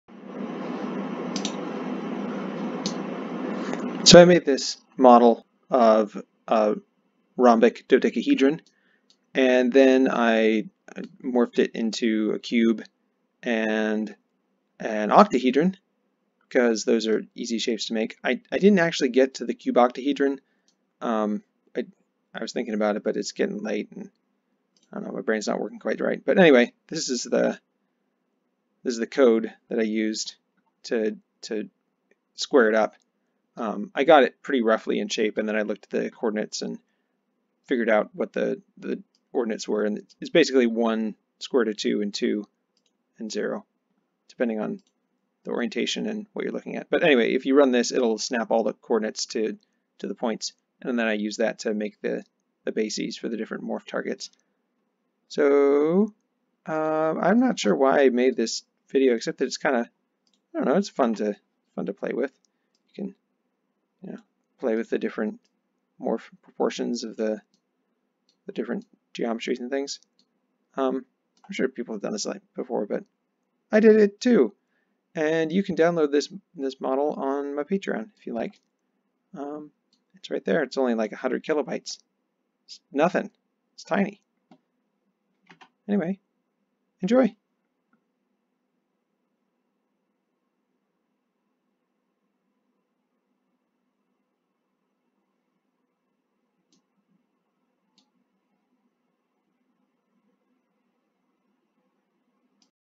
Morphing Rhombic Dodecahedron
I made a Catalan solid in blender that morphs into two of the Platonic solids. Namely, the Rhombic Dodecahedron, Cube, and Octahedron.
Commission your own 3D models at 3D.Tryop.com